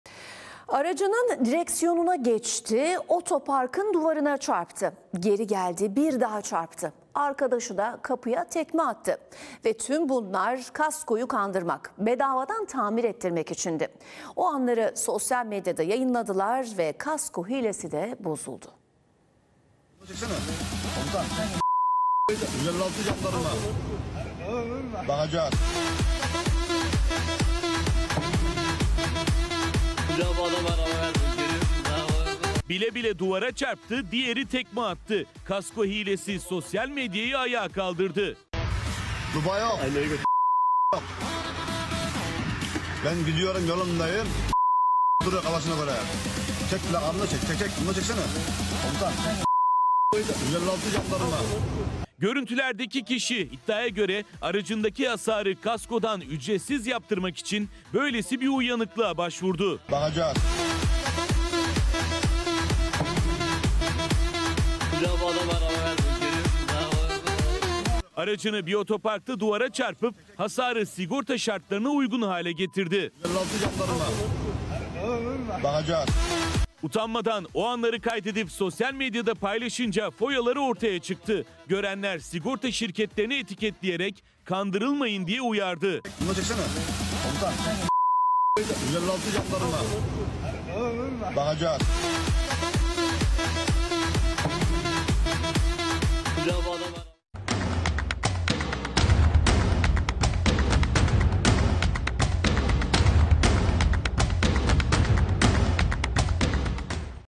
"Kasko Hilesi" cep telefonu kamerasına yansıdı!
Bedava kaskodan yararlanmak için aracının direksiyonuna geçip aracını bilerek duvara çarptı. O anları sosyal medyalarında yayınlayan araç sahipleri hayal ettikleri bedava kaskoya ulaşamadı!